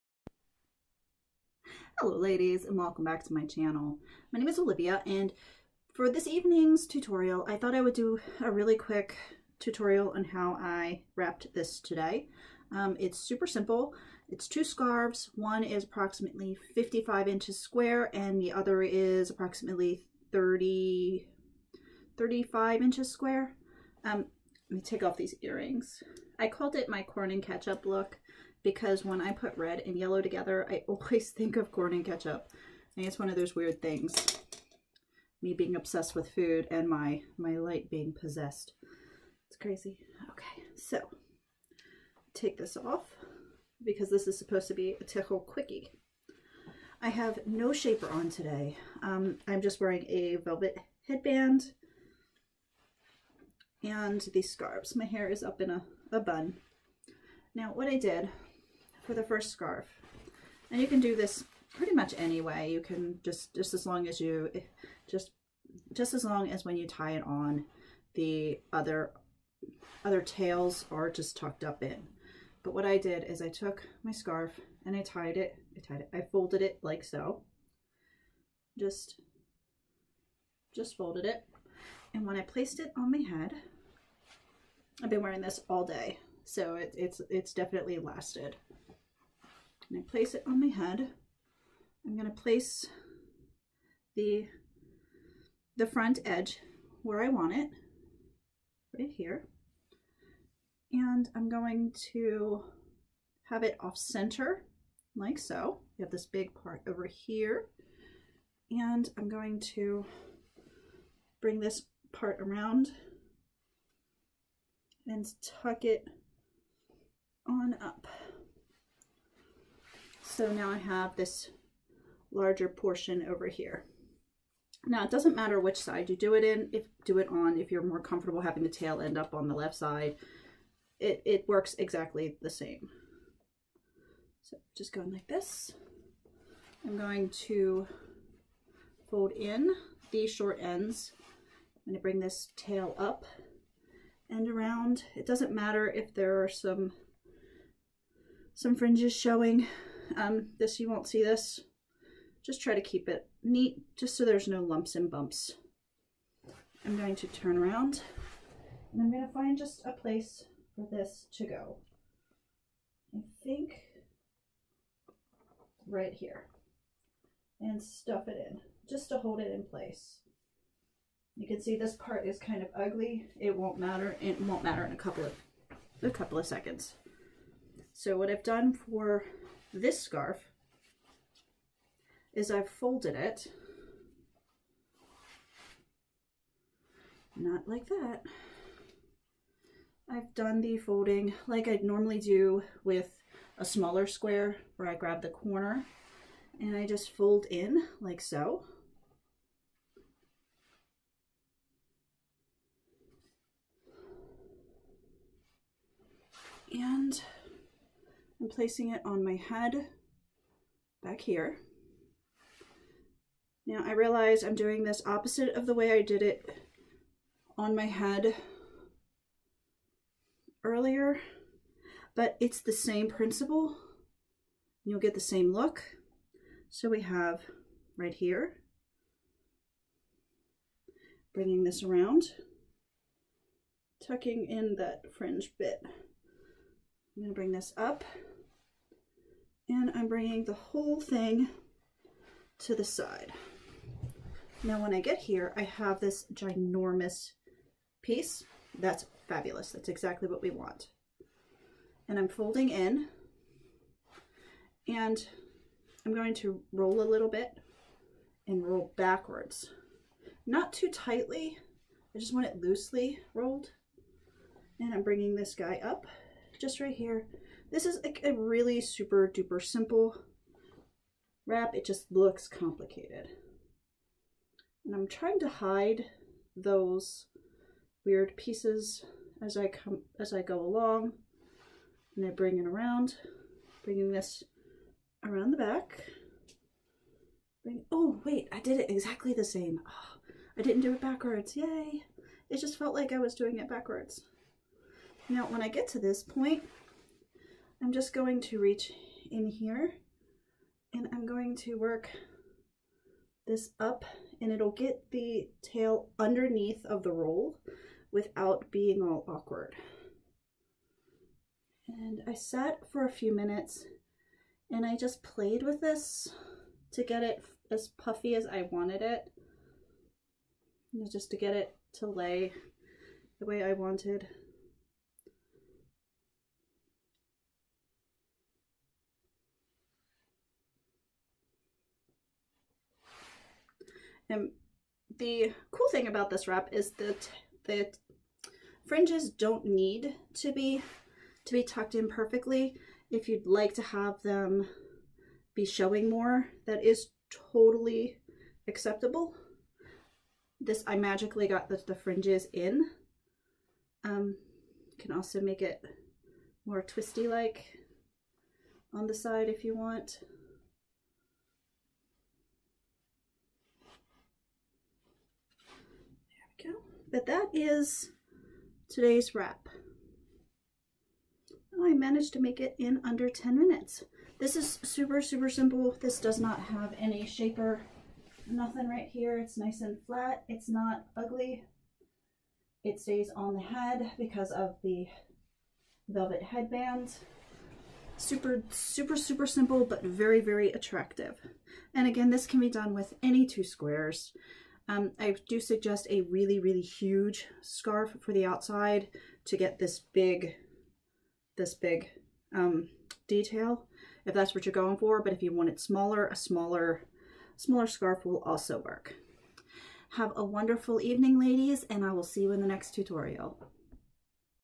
Corn and Ketchup Wrap...a tichel quickie
I know this sounds weird... but when yellow and red are pug together I think of corn and Ketchup.

Now to be serious... I decided to go sans shaper today and am simply wearing a velvet headband. My scarves are both squares. The red being approximately 35” and the other one is significantly larger at  approximately 55”.

Ukrainian scarf